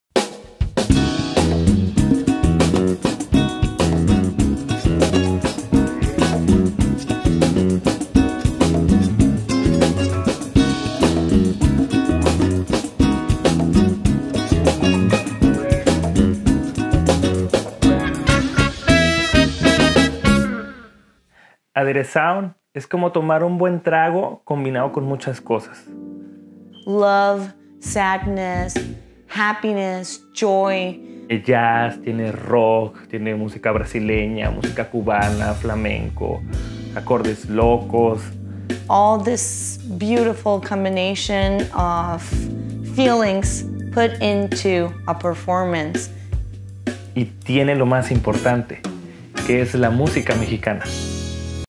Adderesound EPK part 1
Adderesound is the first musical group self-catalogued as Mexican Fusion comprised of top musicians from Mexico and New York City.

From enhancing “Las inmortales de la epoca de oro” (“The inmortal songs of the golden age”) of Hispano-American countries with refreshing arrangements, Adderesound’s original compositions -from ballads, chilenas from Guerrero and norteñas- portray different settings and take the listeners to different regions and times of Mexico and other parts of the World.

Adderesound renders a tribute to the richness of Mexican folklore and creates a bond with different music of other nations that add to the unique sound of the band.